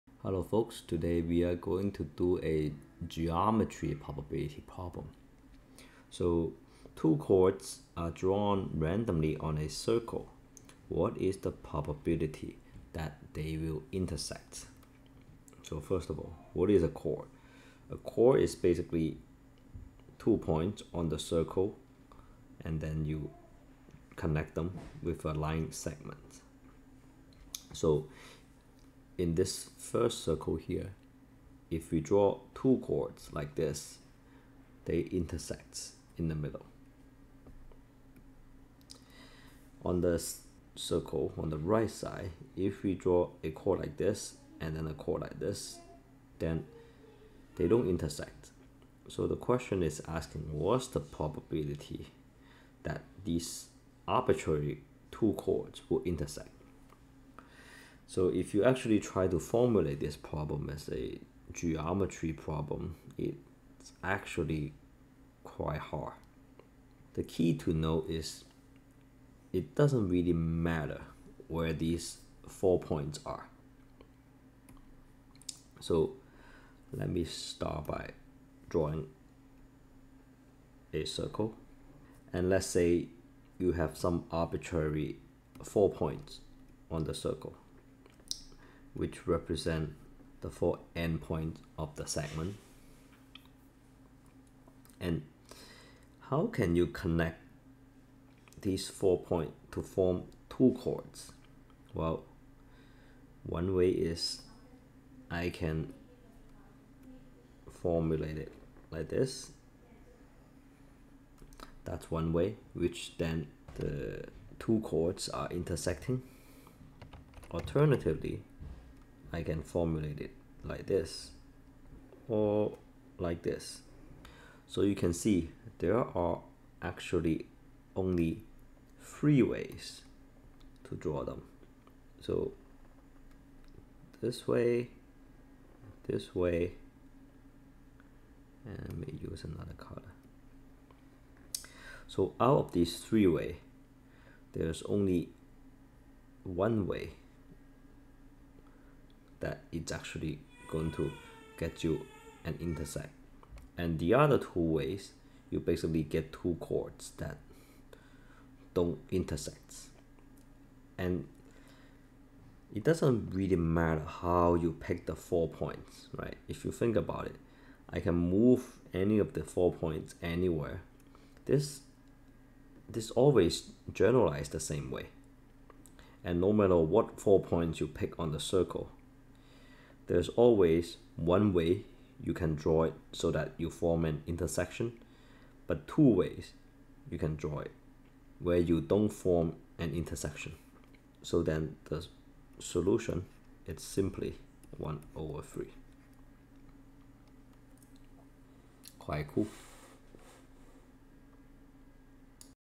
[Probability Interview 10] Two chords on a circle
What is the probability that two chrods on a circle intersect?